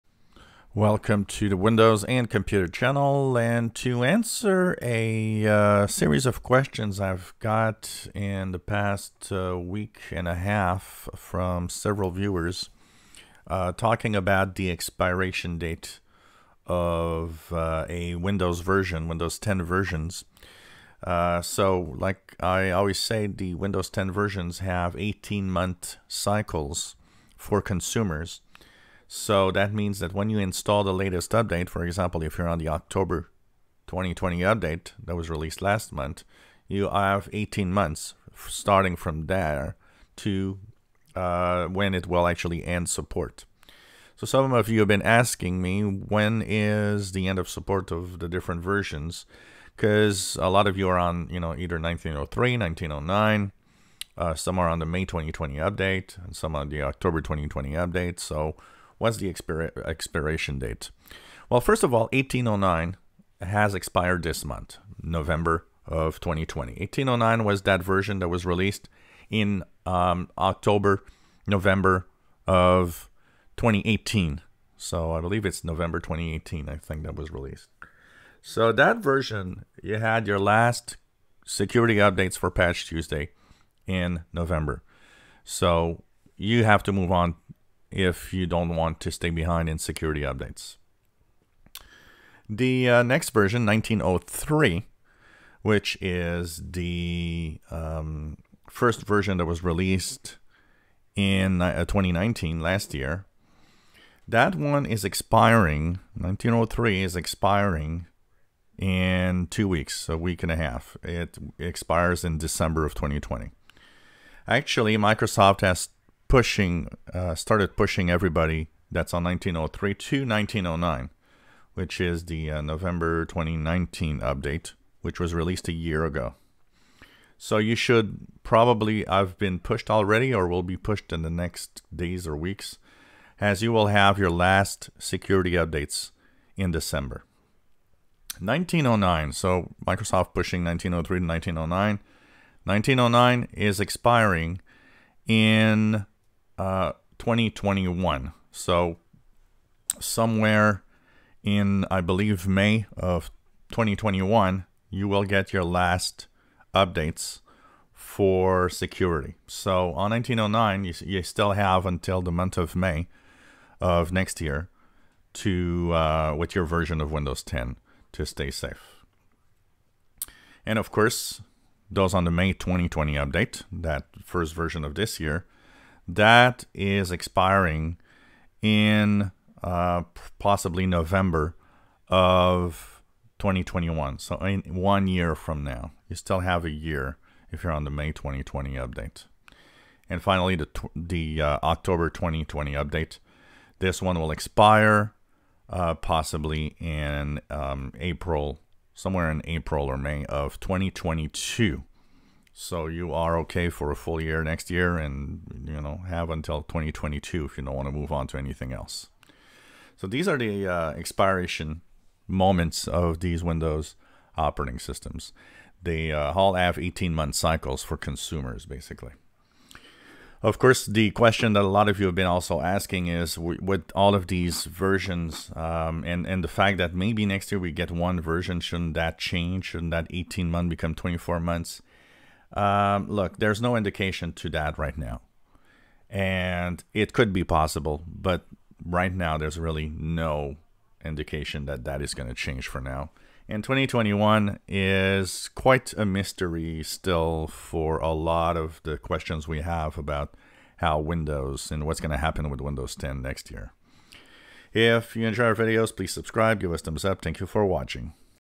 Windows 10 Versions Expiration dates and 18 month cycle November 30th 2020
Windows 10 has a 18 month cycle, here I explain the expiration dates of the supported versions